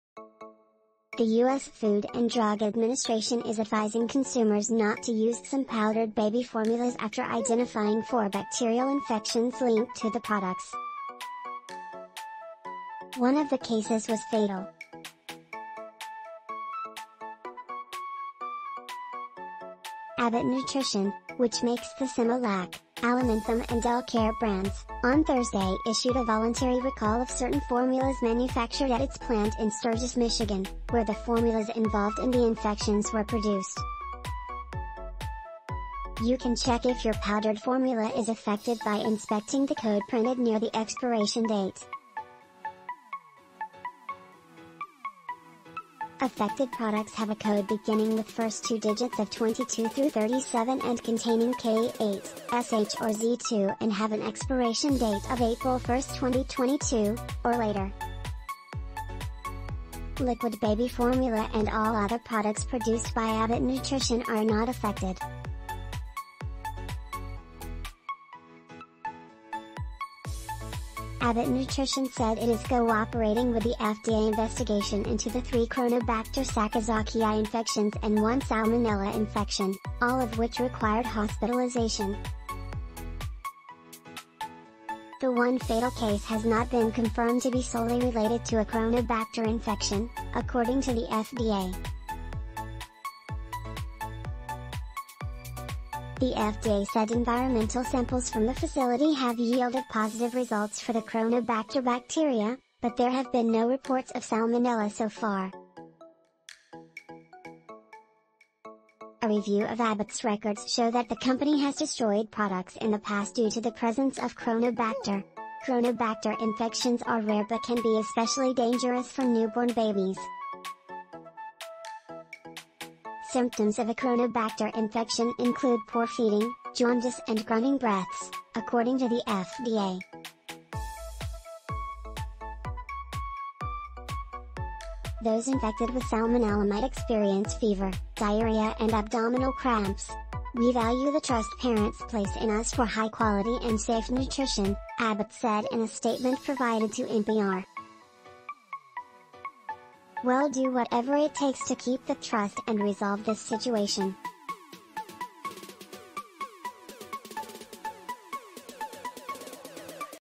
Stop using these baby formulas the FDA says after 4 infants are
The U. S. Food and Drug Administration is advising consumers not to use some powdered baby formulas after identifying four bacterial infections linked to the products. One of the cases was fatal. Abbott Nutrition, which makes the Similac, Alimentum and EleCare brands, on Thursday issued a voluntary recall of certain formulas manufactured at its plant in Sturgis, Mich., where the formulas involved in the infections were produced. You can check if your powdered formula is affected by inspecting the code printed near the expiration date. Affected products have a code beginning with first two digits of 22 through 37 and containing K8, SH or Z2 and have an expiration date of April 1, 2022, or later. Liquid baby formula and all other products produced by Abbott Nutrition are not affected. Abbott Nutrition said it is cooperating with the FDA investigation into the three Cronobacter sakazakii infections and one salmonella infection, all of which required hospitalization. The one fatal case has not been confirmed to be solely related to a Cronobacter infection, according to the FDA. The FDA said environmental samples from the facility have yielded positive results for the Cronobacter bacteria, but there have been no reports of salmonella so far. A review of Abbott's records show that the company has destroyed products in the past due to the presence of Cronobacter. Cronobacter infections are rare but can be especially dangerous for newborn babies. Symptoms of a Cronobacter infection include poor feeding, jaundice and grunting breaths, according to the FDA. Those infected with salmonella might experience fever, diarrhea and abdominal cramps."We value the trust parents place in us for high quality and safe nutrition," Abbott said in a statement provided to NPR. "We'll do whatever it takes to keep that trust and resolve this situation."